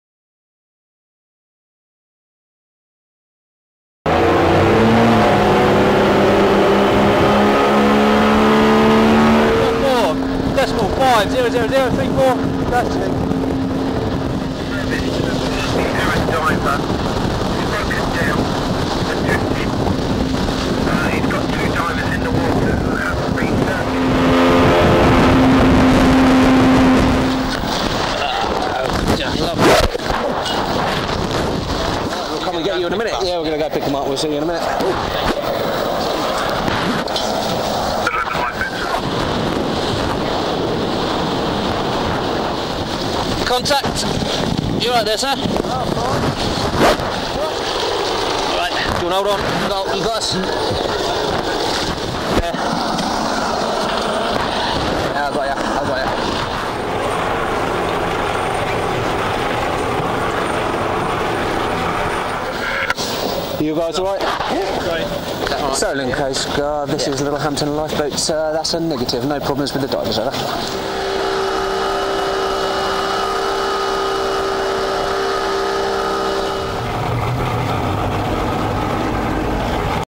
RNLI Littlehampton Lifeboat assisting a Dive Boat
RNLI Littlehampton Lifeboat, assisting Dive Boat with 2 unsurfaced divers. 23rd June 2010.